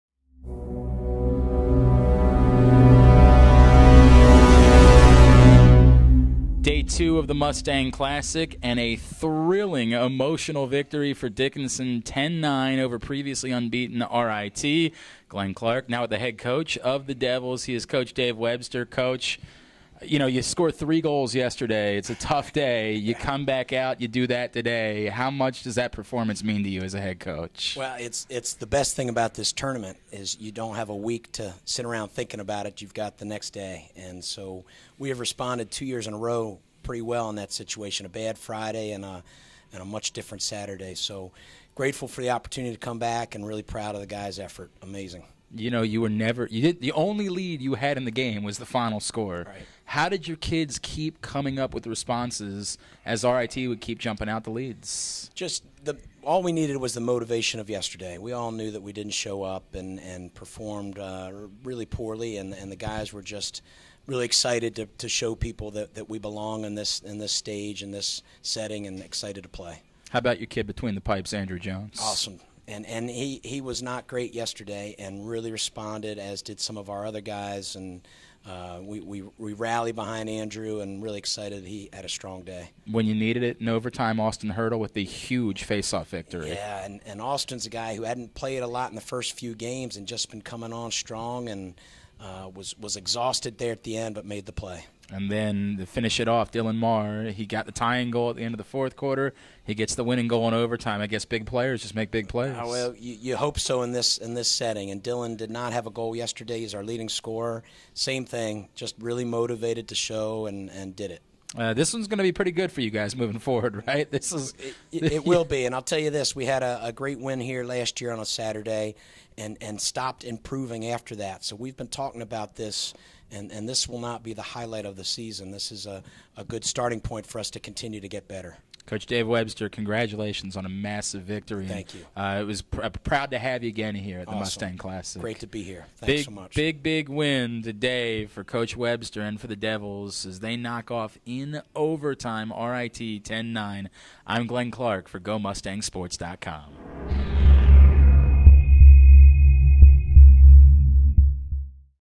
2017 Mustang Classic RIT vs Dickinson Postgame with Dave Webster
Dickinson head coach Dave Webster talks about the team's thrilling overtime victory over RIT during the 2017 Mustang Classic.